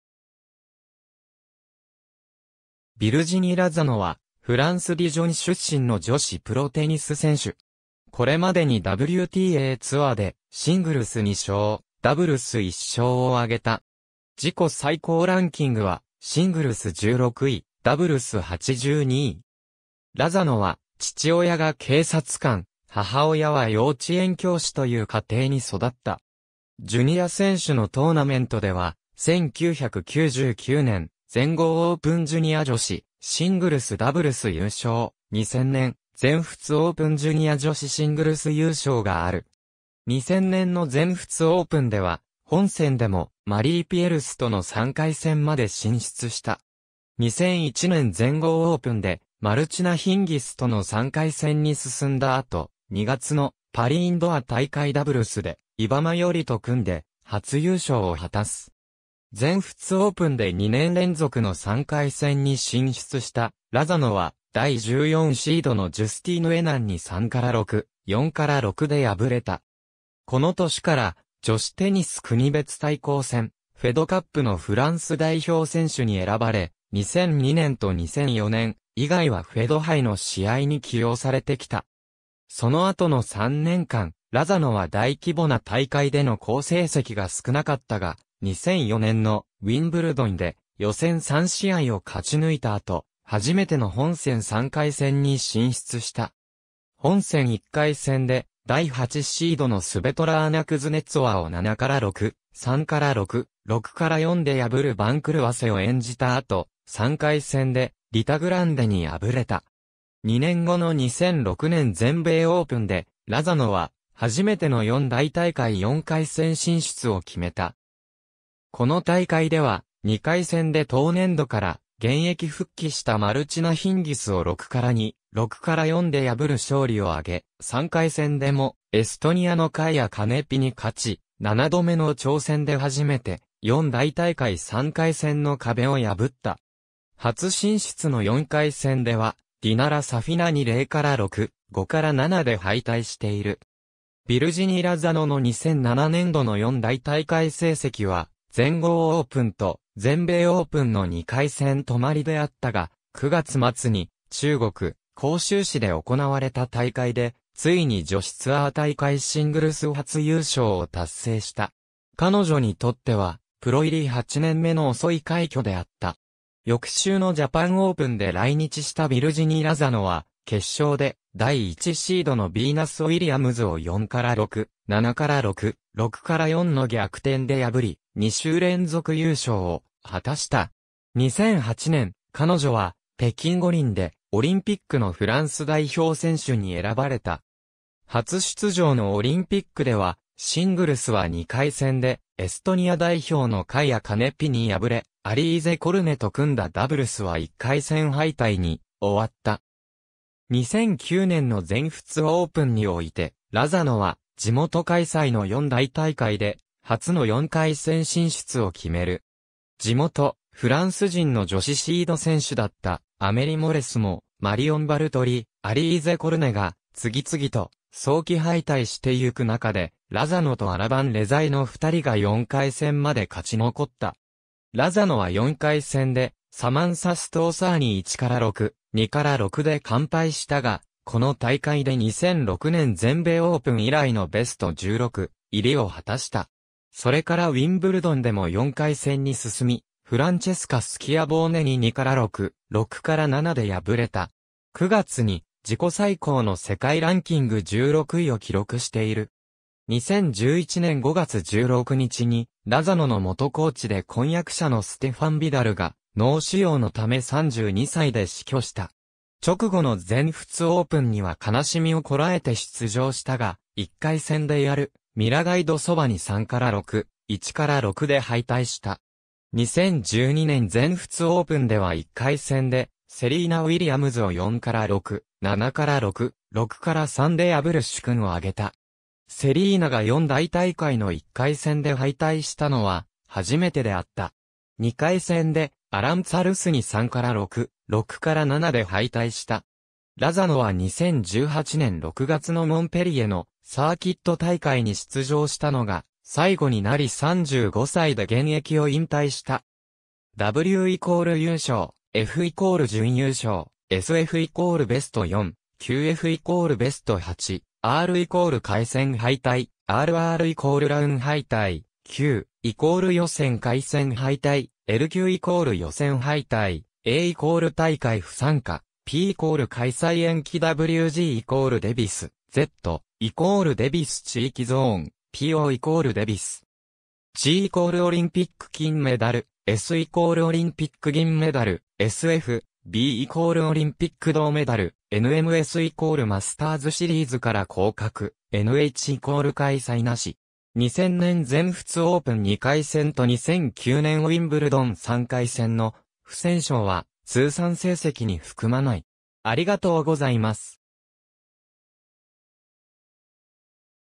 ビルジニ・ラザノ
ビルジニ・ラザノ（Virginie Razzano, 1983年5月12日 - ）は、フランス・ディジョン出身の女子プロテニス選手。
これまでにWTAツアーでシングルス2勝、ダブルス1勝を挙げた。
ラザノは父親が警察官、母親は幼稚園教師という家庭に育った。
ジュニア選手のトーナメントでは、1999年全豪オープンジュニア女子シングルス・ダブルス優勝、2000年全仏オープンジュニア女子シングルス優勝がある。
2000年の全仏オープンでは、本戦でもマリー・ピエルスとの3回戦まで進出した。
2001年全豪オープンでマルチナ・ヒンギスとの3回戦に進んだ後、2月の「パリ・インドア」大会ダブルスでイバ・マヨリと組んで初優勝を果たす。
全仏オープンで2年連続の3回戦に進出したラザノは、第14シードのジュスティーヌ・エナンに 3-6, 4-6 で敗れた。
この年から女子テニス国別対抗戦・フェドカップのフランス代表選手に選ばれ、2002年と2004年以外はフェド杯の試合に起用されてきた。
その後の3年間、ラザノは大規模な大会での好成績が少なかったが、2004年のウィンブルドンで予選3試合を勝ち抜いた後、初めての本戦3回戦に進出した。
本戦1回戦で第8シードのスベトラーナ・クズネツォワを 7-6, 3-6, 6-4 で破る番狂わせを演じた後、3回戦でリタ・グランデに敗れた。
2年後の2006年全米オープンで、ラザノは初めての4大大会4回戦進出を決めた。
この大会では、2回戦で当年度から現役復帰したマルチナ・ヒンギスを 6-2, 6-4 で破る勝利を挙げ、3回戦でもエストニアのカイア・カネピに勝ち、7度目の挑戦で初めて4大大会3回戦の壁を破った。
初進出の4回戦ではディナラ・サフィナに 0-6, 5-7 で敗退している。
ビルジニ・ラザノの2007年度の4大大会成績は、全豪オープンと全米オープンの2回戦止まりであったが、9月末に中国・広州市で行われた大会でついに女子ツアー大会シングルス初優勝を達成した。
彼女にとっては、プロ入り8年目の遅い快挙であった。
翌週のジャパン・オープンで来日したビルジニ・ラザノは、決勝で第1シードのビーナス・ウィリアムズを 4-6, 7-6, 6-4 の逆転で破り、2週連続優勝を果たした。
2008年、彼女は北京五輪でオリンピックのフランス代表選手に選ばれた。
初出場のオリンピックでは、シングルスは2回戦でエストニア代表のカイア・カネピに敗れ、アリーゼ・コルネと組んだダブルスは1回戦敗退に終わった。
2009年の全仏オープンにおいて、ラザノは地元開催の4大大会で初の4回戦進出を決める。
地元フランス人の女子シード選手だったアメリ・モレスモ、マリオン・バルトリ、アリーゼ・コルネが次々と早期敗退してゆく中で、ラザノとアラバン・レザイの2人が4回戦まで勝ち残った。
ラザノは4回戦でサマンサ・ストーサーに 1-6, 2-6 で完敗したが、この大会で2006年全米オープン以来のベスト16入りを果たした。
それからウィンブルドンでも4回戦に進み、フランチェスカ・スキアボーネに 2-6, 6-7 で敗れた。
9月に自己最高の世界ランキング16位を記録している。
2011年5月16日にラザノの元コーチで婚約者のステファン・ヴィダルが、脳腫瘍のため32歳で死去した。
直後の全仏オープンには悲しみを堪えて出場したが、1回戦でヤルミラ・ガイドソバに 3-6, 1-6 で敗退した。
2012年全仏オープンでは1回戦でセリーナ・ウィリアムズを 4-6, 7-6, 6-3 で破る殊勲を挙げた。
セリーナが4大大会の1回戦で敗退したのは初めてであった。
2回戦でアランツァ・ルスに 3-6, 6-7 で敗退した。
ラザノは2018年6月のモンペリエのサーキット大会に出場したのが最後になり35歳で現役を引退した。
W=優勝, F=準優勝, SF=ベスト4, QF=ベスト8, R=回戦敗退, RR=ラウンドロビン敗退, Q#=予選#回戦敗退, LQ=予選敗退, A=大会不参加, P=開催延期 WG=デビスカップワールドグループ, Z#=デビスカップ地域ゾーン, PO=デビスカッププレーオフ,
G=オリンピック金メダル, S=オリンピック銀メダル, SF-B=オリンピック銅メダル...